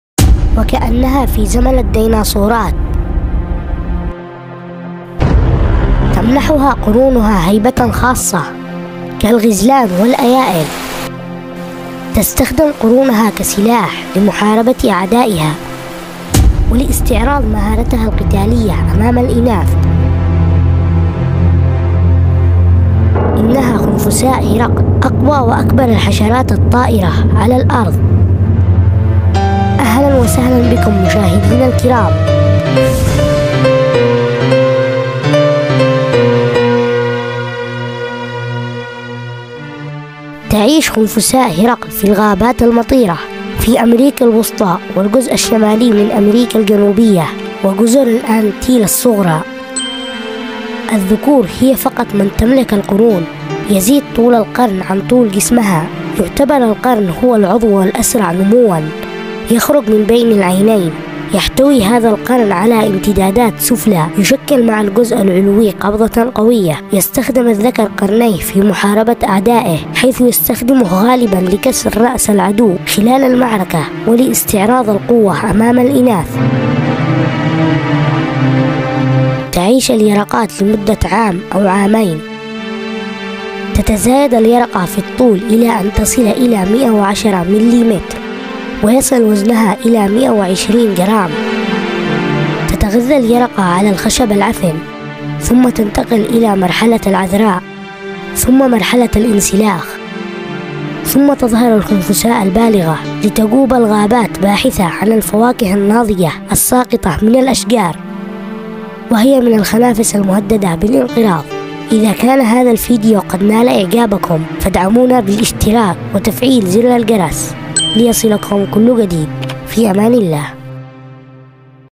خنفساء هرقل اقوى واكبر الحشرات الطائرة على الأرض
خنفساء هرقل من أقوي وأكبر الخنافس الطائرة على وجه الأرض  قوية الى درجة انها تستطيع أن تحمل 850ضعف وزنها .قرونها أطول من جسمها  تستخدم قرونها كا كماشة ويعتبر كا سلاح فتاك لمحاربة اعدائها ولي استعراض المهارات القتالية أمام الإناث .لا تمتلك الإناث قرون الذكور فقط لديهم  قرون
أتمنى أن ينال إعجابكم
لا تنسو دعمنا باالإشتراك  بالقناة وتفعيل زر الجرس تشجيعًا لنا لنستمر معكم